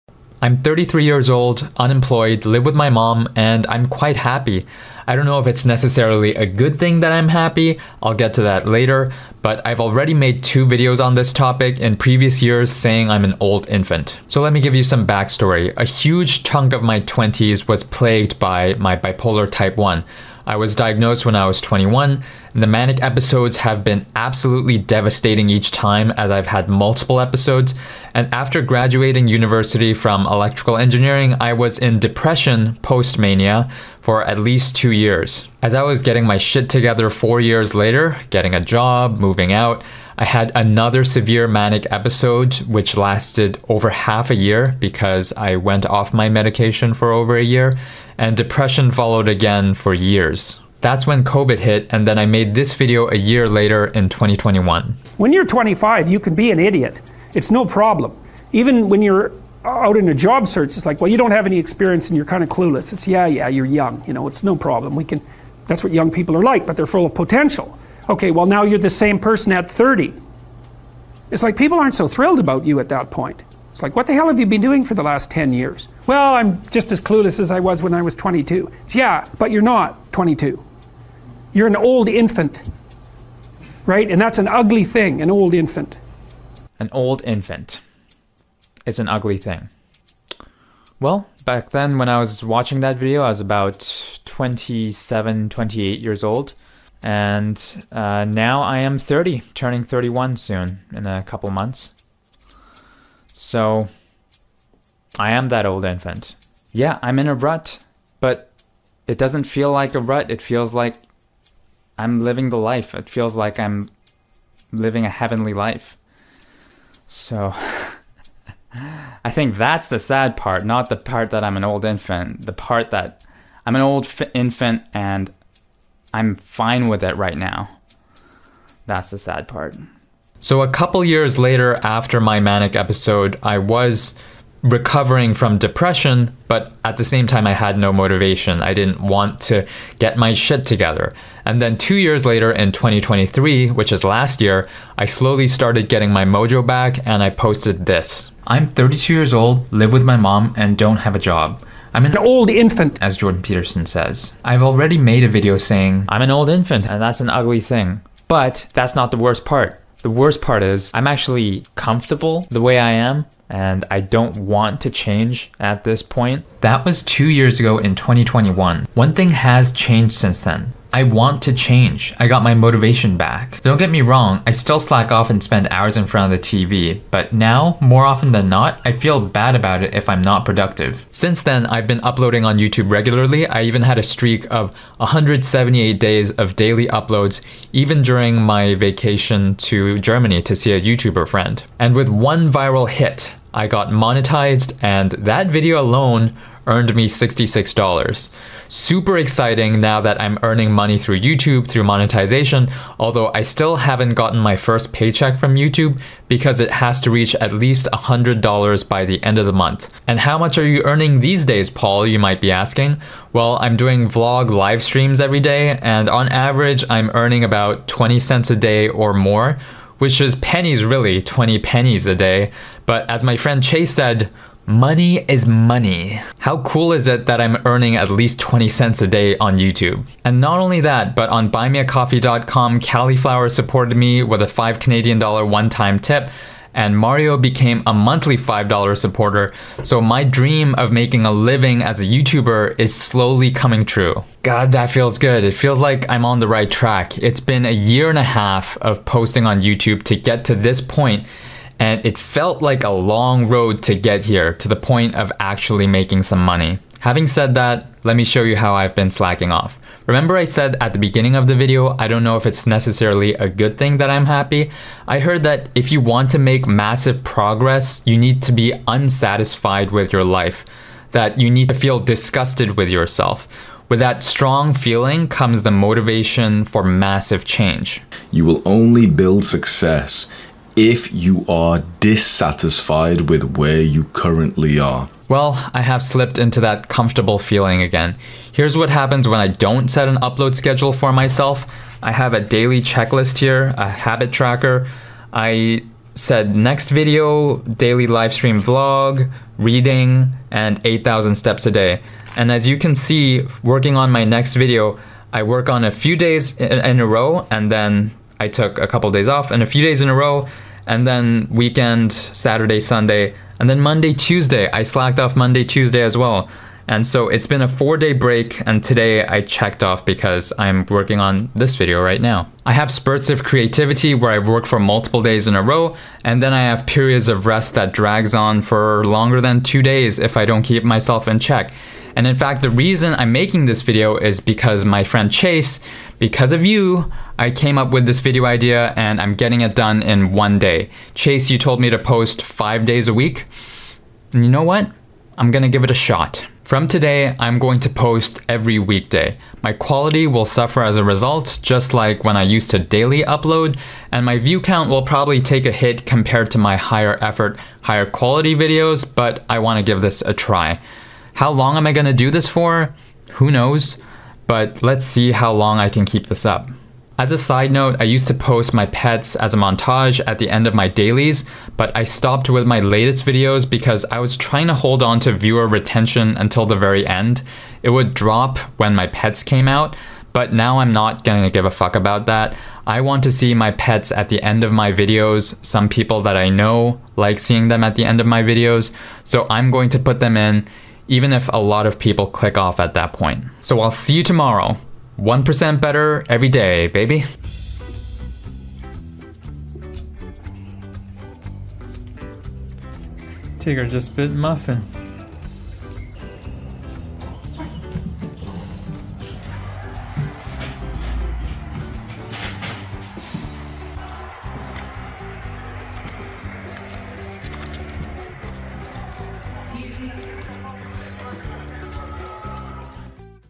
Unemployed bipolar 33-year-old, I live with my mom, I’m happy
Here's to starting daily weekday uploads!

0:00 video
6:37 pet montage